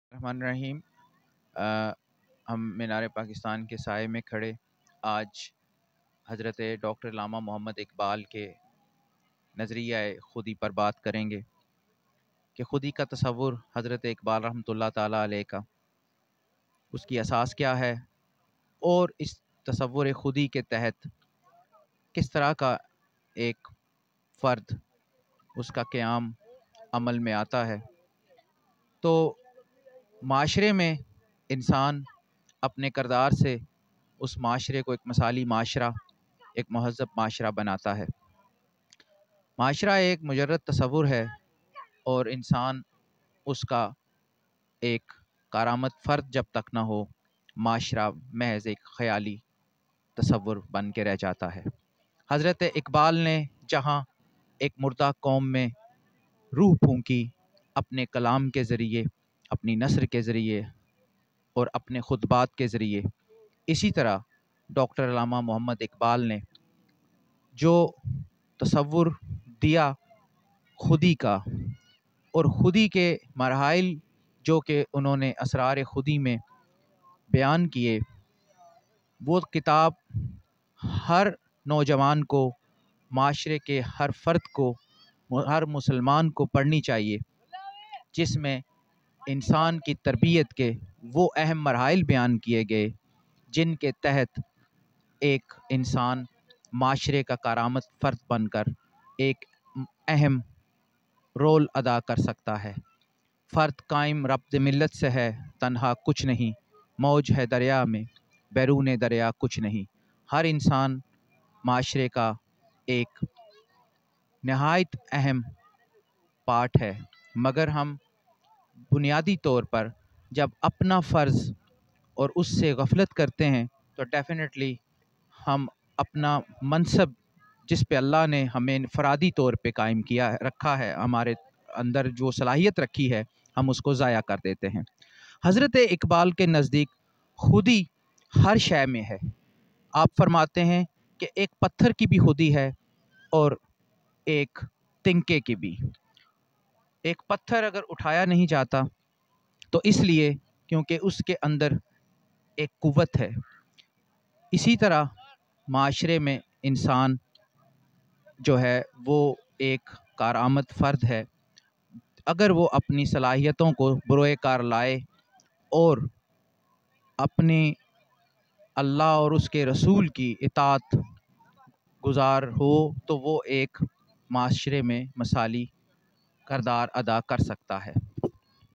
Man Masculinity and Dr.Allama Muhammad Iqbal || Theory of Self || MASCULINITIES | SRAW
Dr.Allama Iqbal's Concept of Khuddi in Israr e Khuddi.